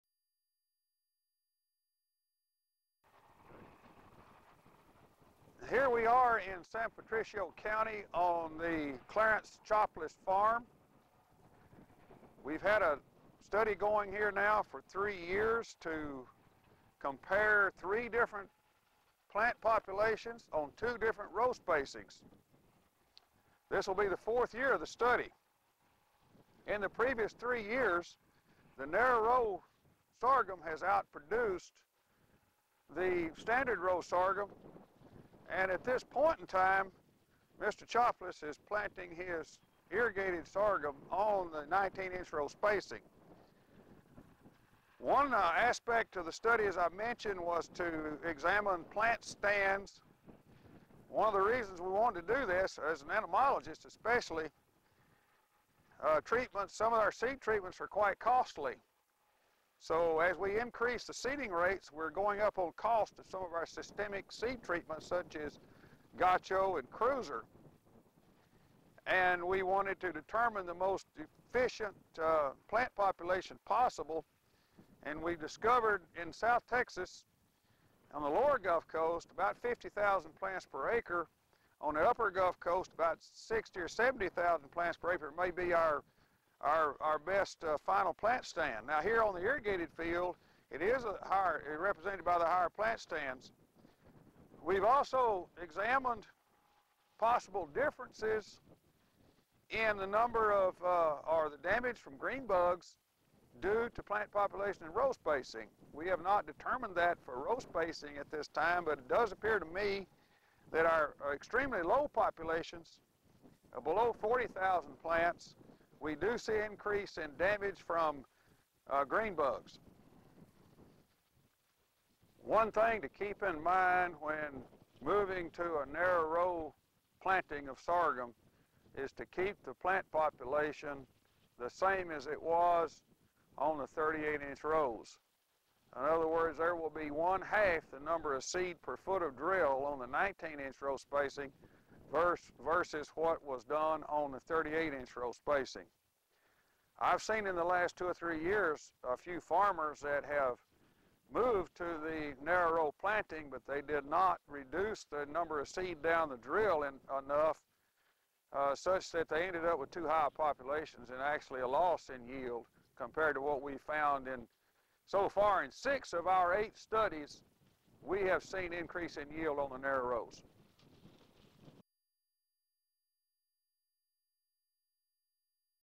Sorghum Row Spacing
Effects of sorghum row spacing on yield and greenbug numbers on the Gulf Coast